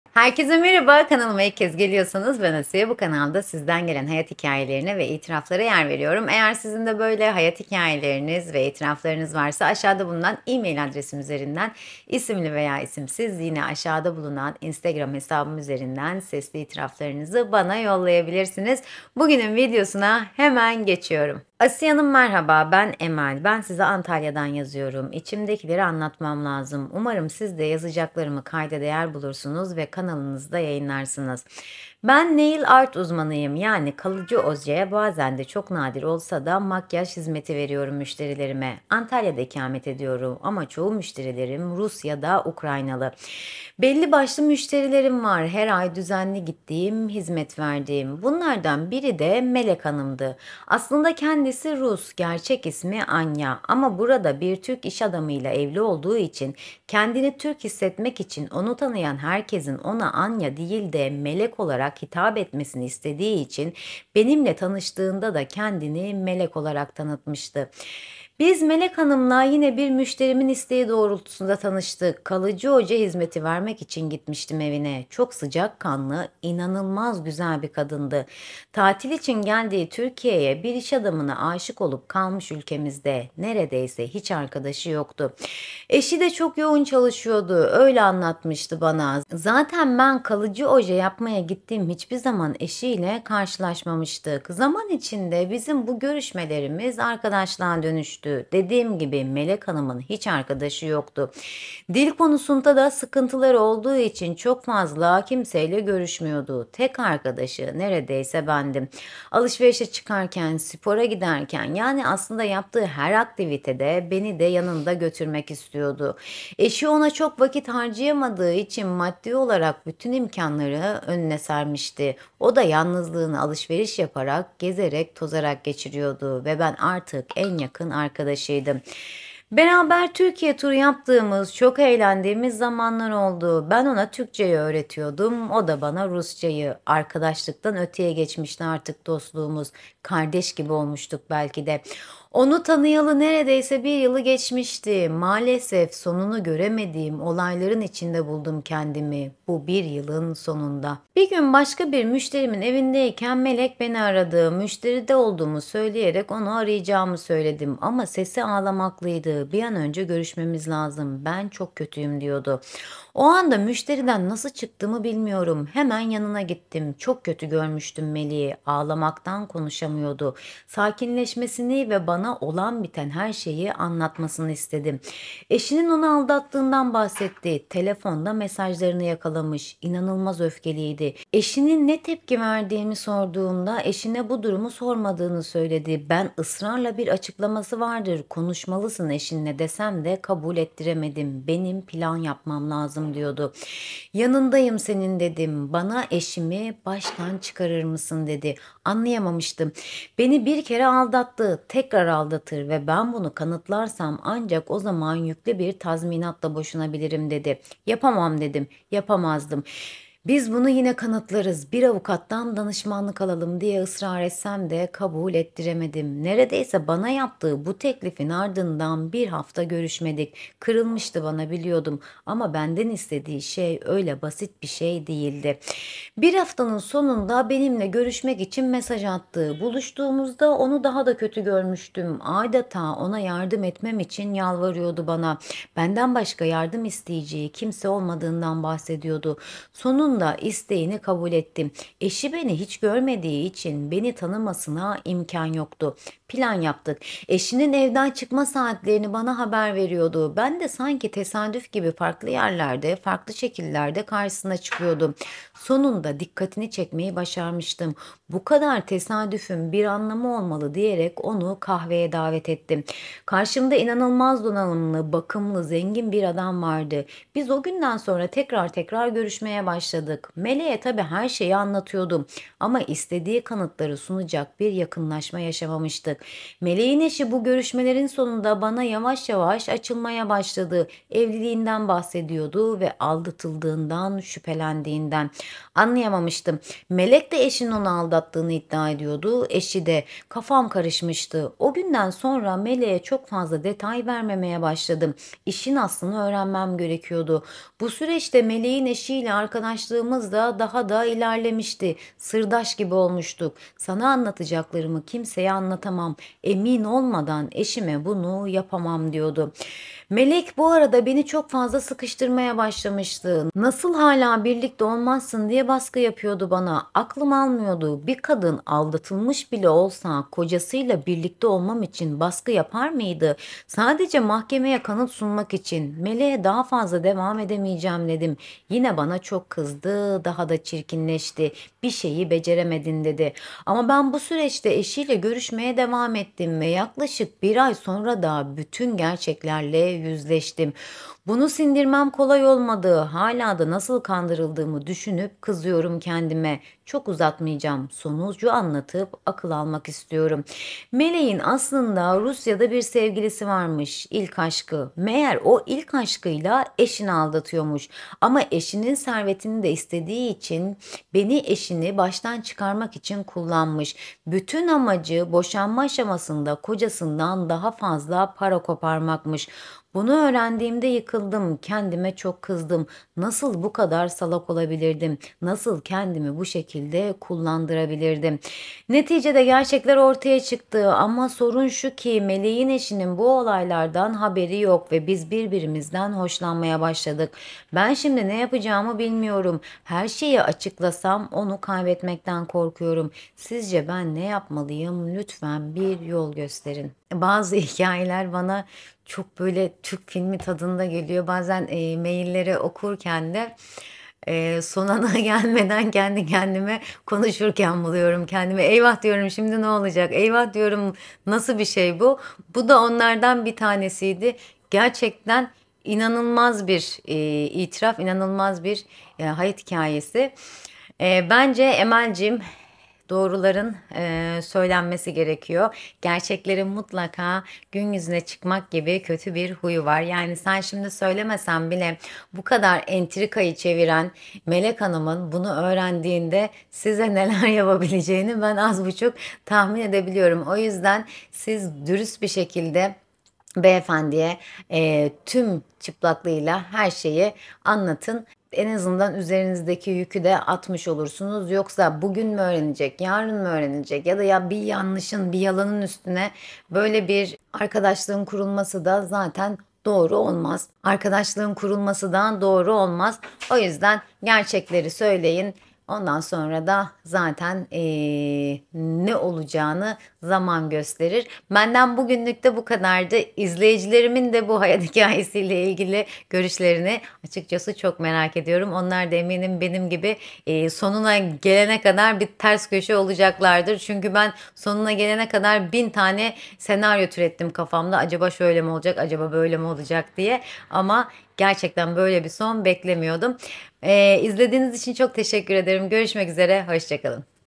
Arkadaşım Kocasını Baştan Çıkarmamı İstedi | İtiraf Vakti
Antalya'da yaşayan izleyicim Nail Art yani tırnak sanatı ile ilgilenir. Bir gün müşterisi zaman içinde en yakın arkadaşı olur. Fakat arkadaşı kocasından şüpheleniyordur. Arkadaşından ilginç bir istek ister; kocamı baştan çıkar.